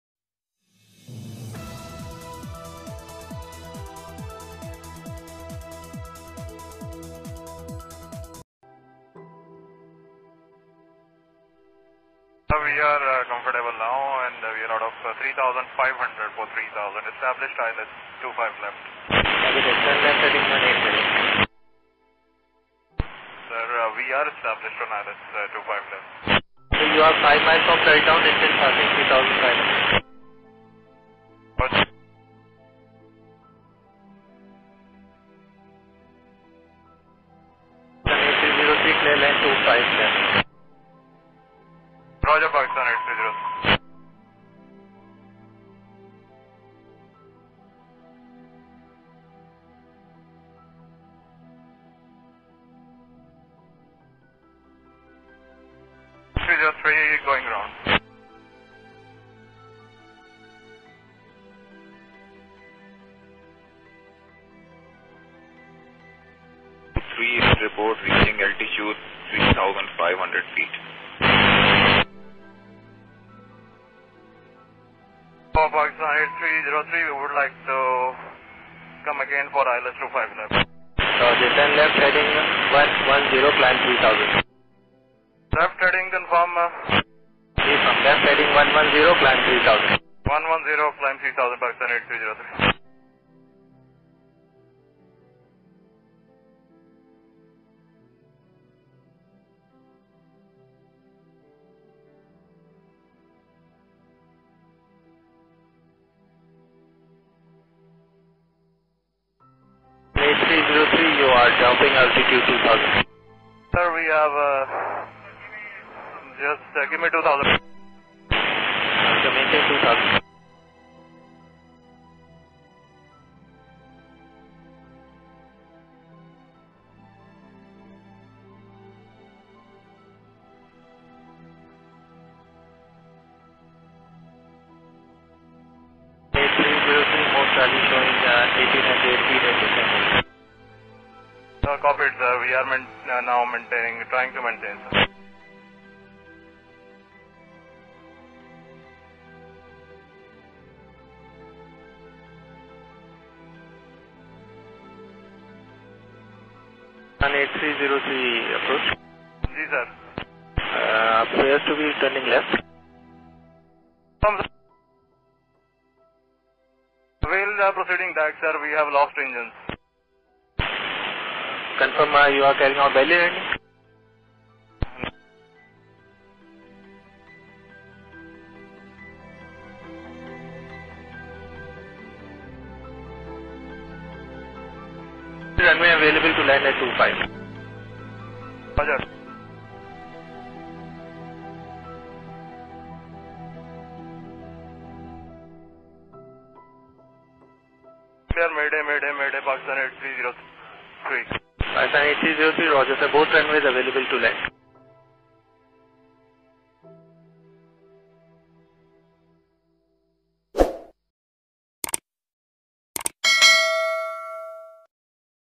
PIA flight 8303 crashes near Karachi,telecommunication between pilot and air traffic control tower..
Audio clip before PK 8303 crash landed in Karachi residential area..

The flight originated from Lahore, The aircraft was registered as AP-BLD. The aircraft was unable to extend it's landing gear and made a go-around at first attempt to land at Karachi airport. The aircraft was having troubles maintaining the proper altitude as obvious from the recordings. On it's second attempt, one minute (1km) away from the runway threshold it lost both of it's engines and the aircraft banked slightly to the left and crashed in the residential area. There were 91 souls on board with 8 crew members. It is said at the time of writing that two people has survived the crash...